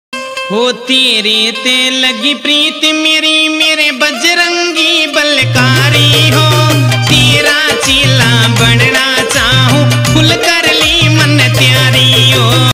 Mehandipur Balaji Bhajan song, Mukesh Sharma Urlaniya latest haryanvi Bhajan song
New Balaji bhajan 2023|mukesh sharma hit Balaji Bhajan|bkd bhajan,